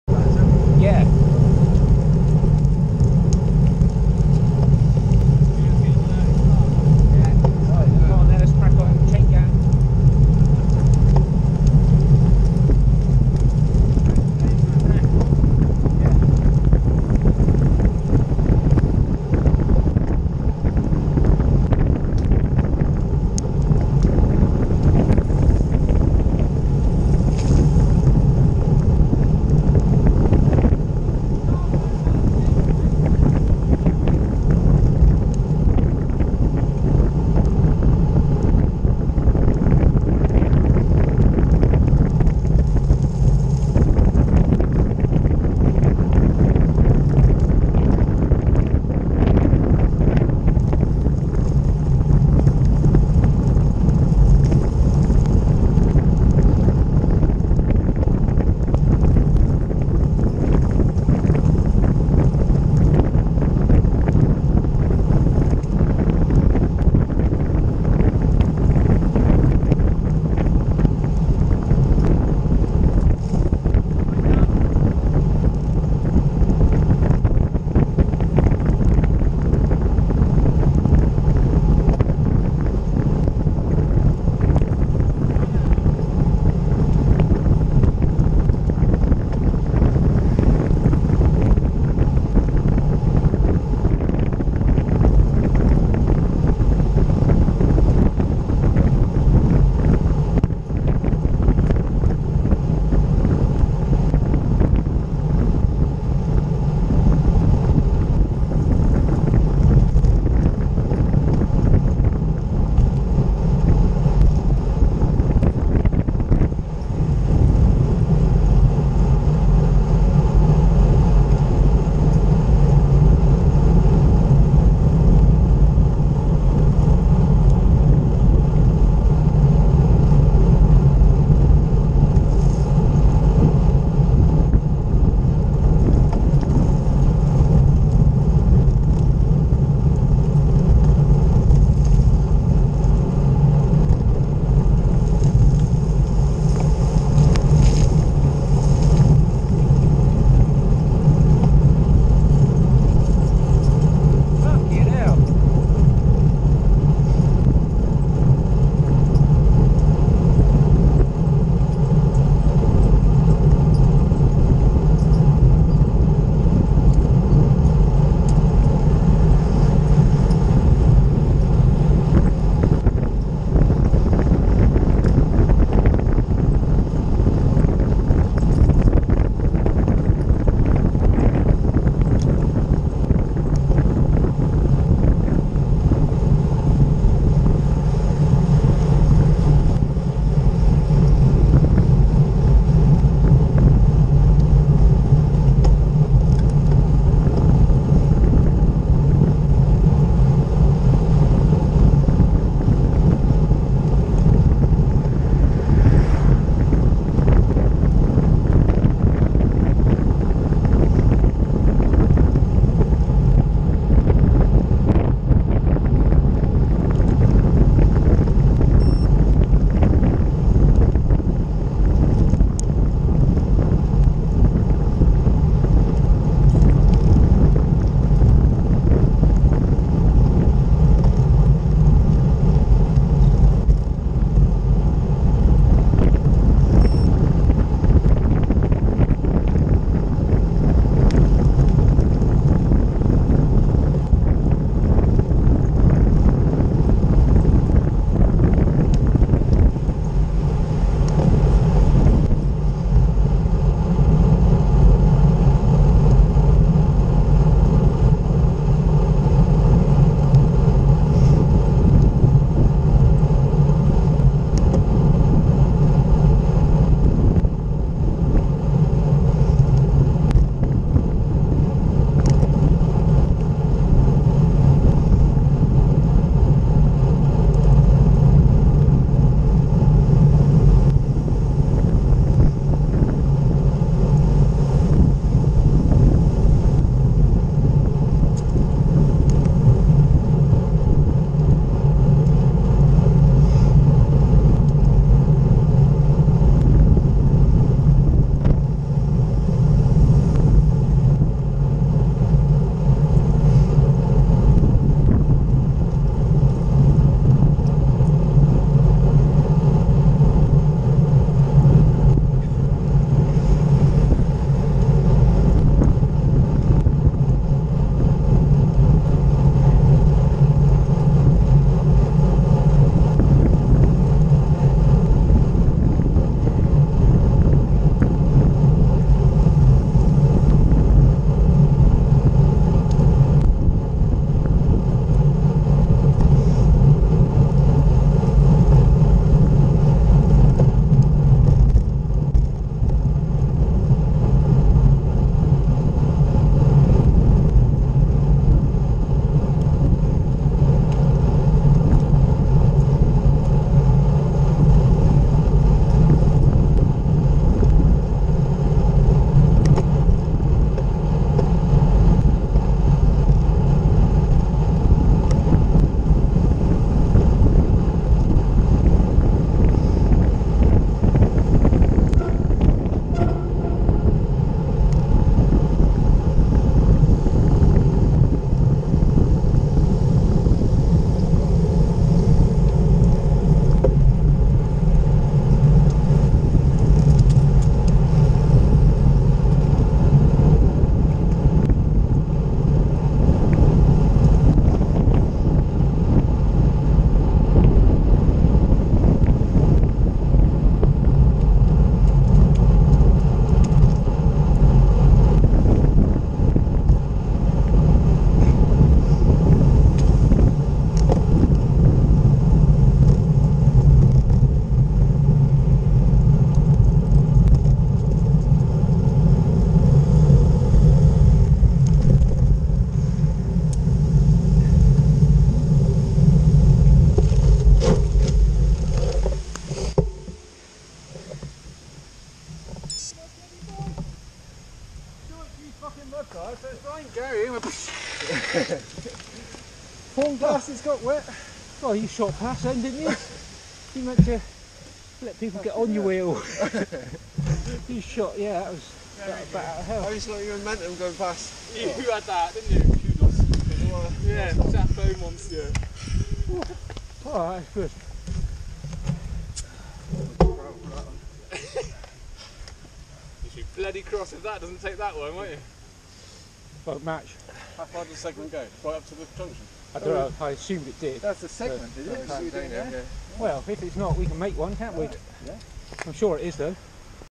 CTC Maiden Bradley to Kingston Deverill chain gang
Chain-ganging it from Maiden Bradley to Kingston Deverill with Frome CTC. Camera gets a bit muddy!